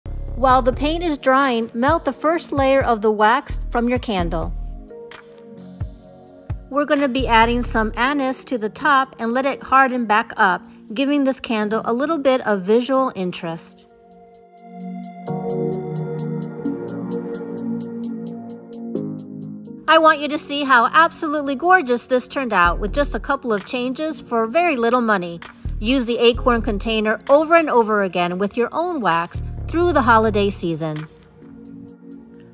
AN ALDI ITEM REVAMP || SETTING THE MOOD
Let’s make it even better. I’ll show you how to take an ALDI fall candle jar and elevate the look with  paint and add something special to the wax for a higher-end look. See the full video - Eek to Chic #8. Its short and super easy. Best of all, affordable!